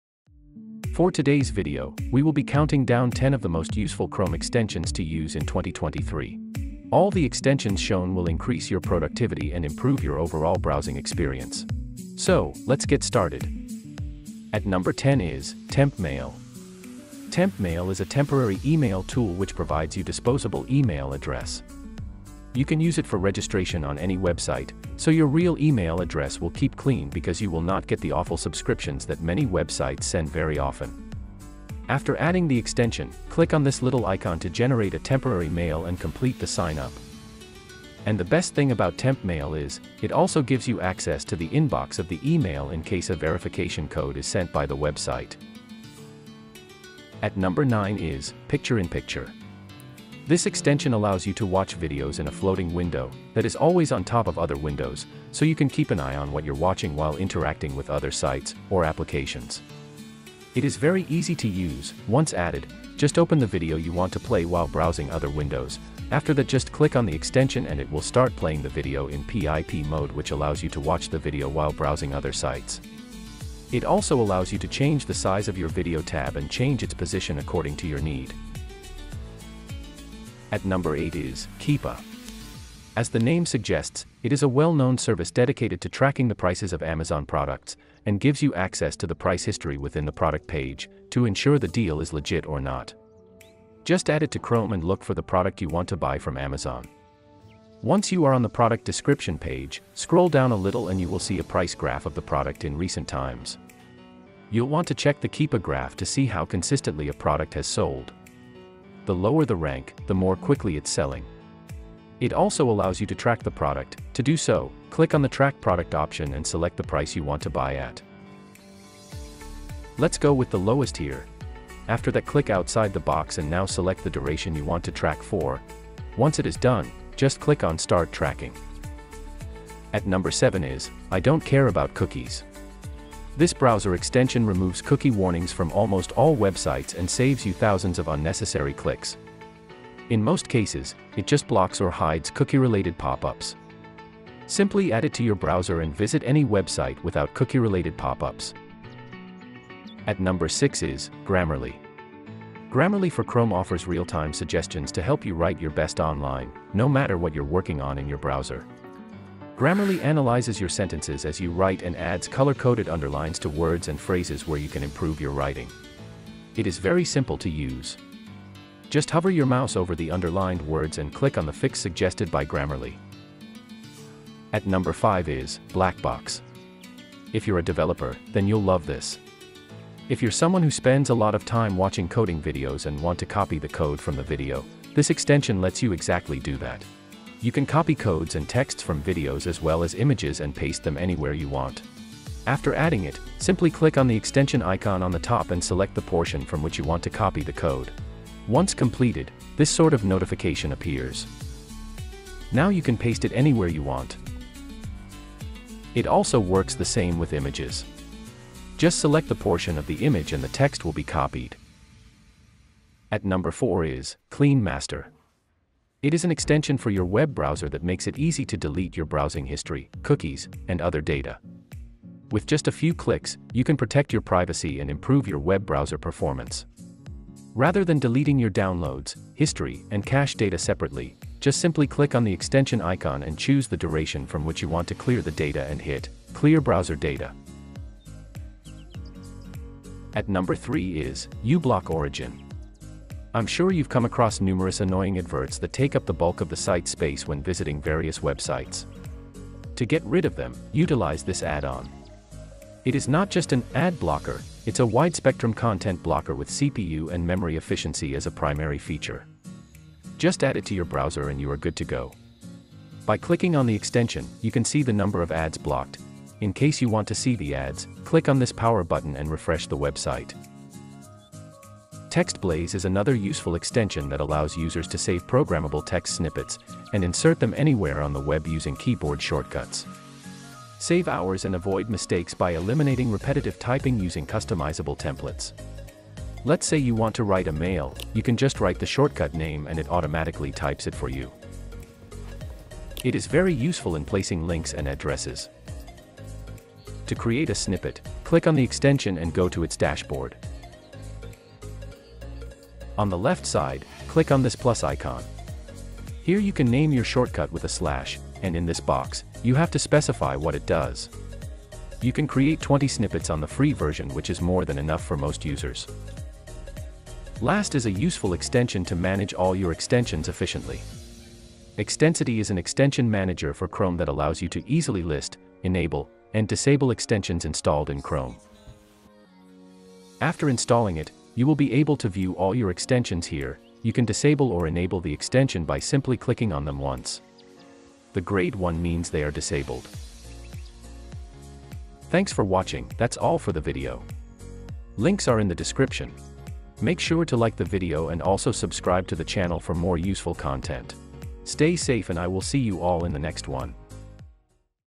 Top 10 Best Chrome Extensions to Use Now! 2023
10 Most Useful Chrome Extensions Everyone Should Know. All the extensions shown will increase your productivity and improve your overall browsing experience.In addition to the Google Chrome Browser, the extensions I show you can also be used in other Chromium-based browsers like Vivaldi, Brave, Opera and the new Microsoft Edge.